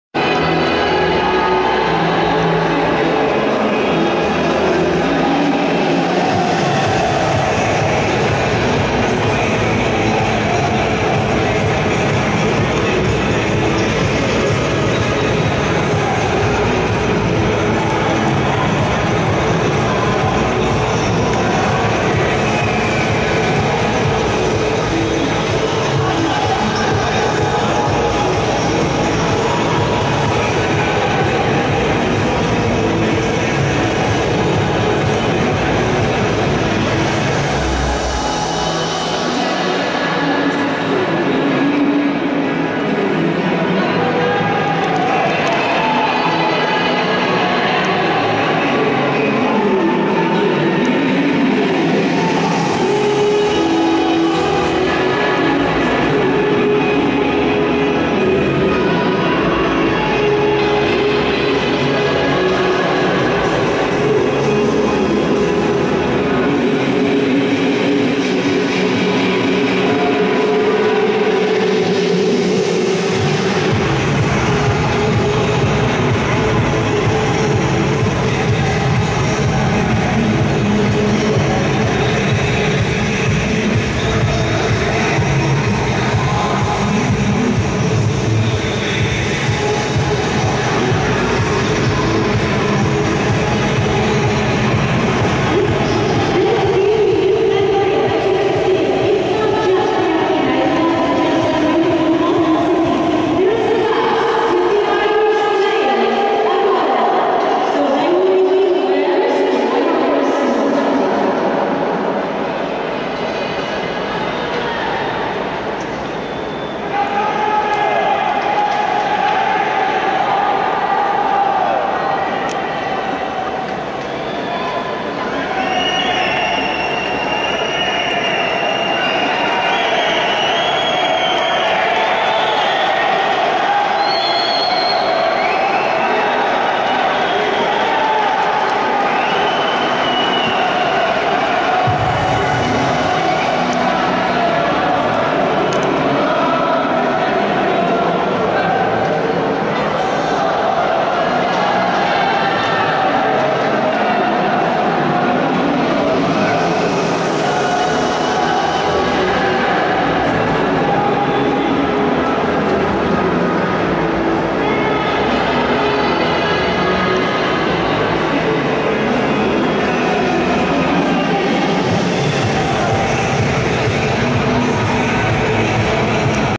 L'Italia_entra_in_campo_a_Poznan_a_90_minuti_dal_calcio_d'inizio18.6.flv
VIDEO ESCLUSIVO DA NON PERDERE. LA NOSTRA NAZIONALE ENTRA IN CAMPO A 90 MINUTI DAL CALCIO D'INIZIO DELLA PARTITA CONTRO L'IRLANDA. ECCO GLI STRISCIONI DEI TIFOSI DI TUTTA ITALIA PRESENTI ALLO STADIO